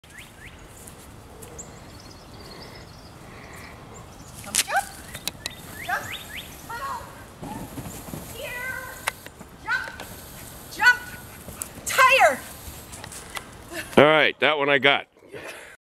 Ken gets the perfect run with the video camera  under control with Lulu.  3/26/11